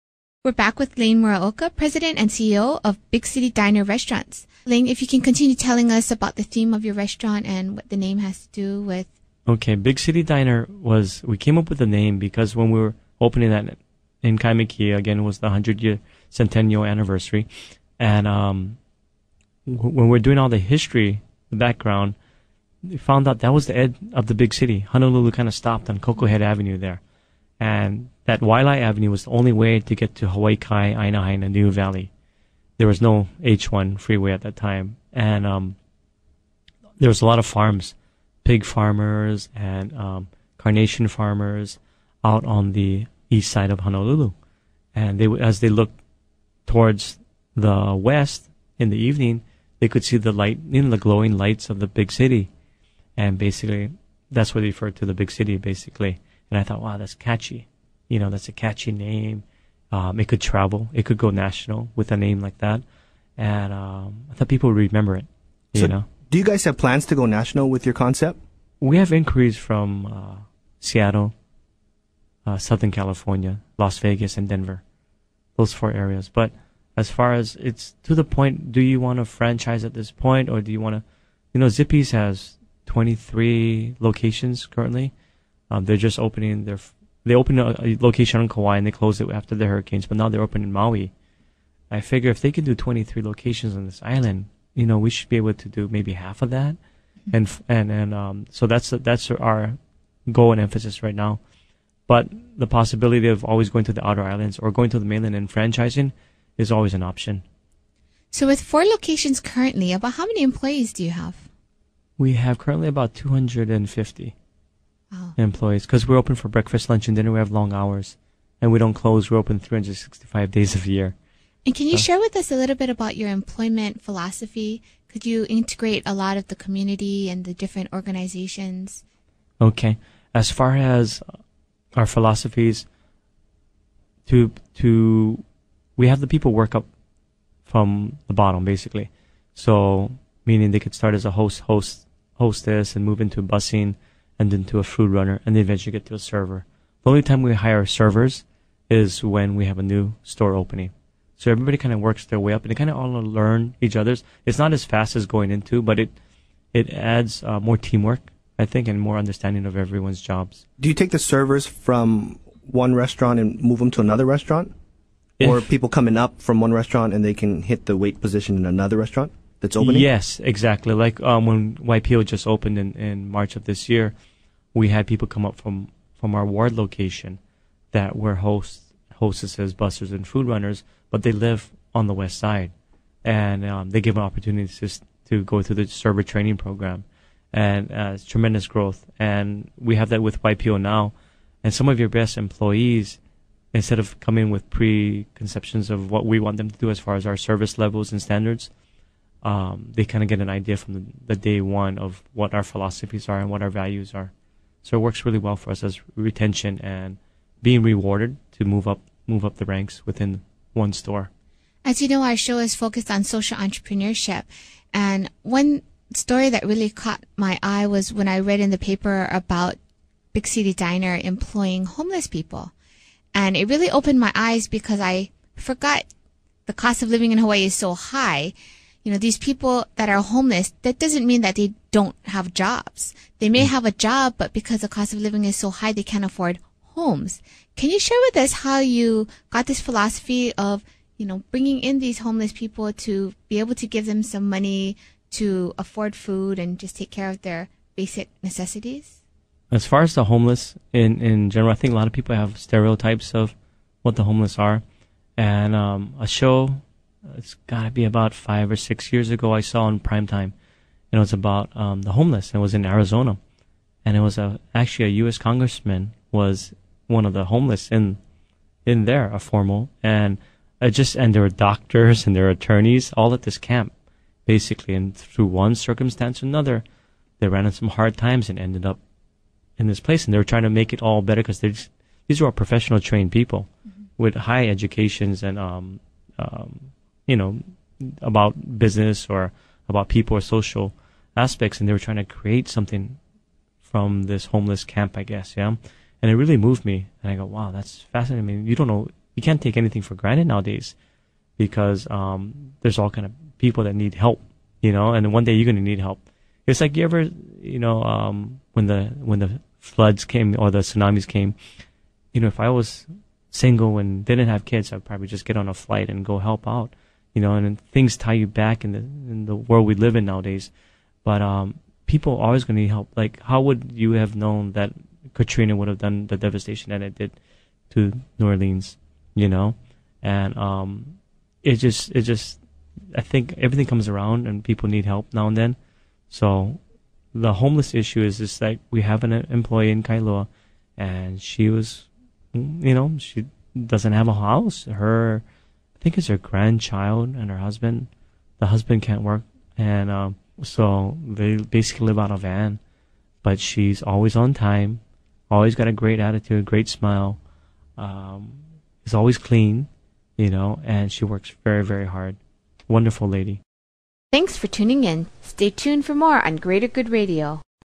LANE MURAOKA - Part 3
LANE MURAOKA - President and CEO of Big City Diner

Lane Muraoka has turned his community minded philosophy into a business with a big heart and big success. Big City Diner has grown into 4 locations and big plans for the future. Big City Diner helps employ, train and support people that have fallen on tough times but are still good people. This was recognized by the Division of Vocational Rehab’s employee and employer of the year award that they won.

Hear Lane discuss how it was to go from being an employee to becoming a business owner. Learn how he determined their key locations and what he thinks is crucial to the success of a restaurant. More importantly, let Lane share his philosophy and winning attitude to get more out of your team and work.